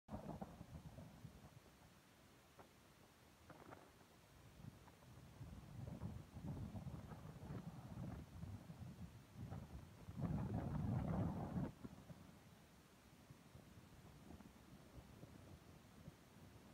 Hermine and Oliver Hazard Perry Meet-cute
Posttropical cyclone Hermine stormed Fort Adams with choppy waves, nudging the tall ship, Oliver Hazard Perry, for most of the morning.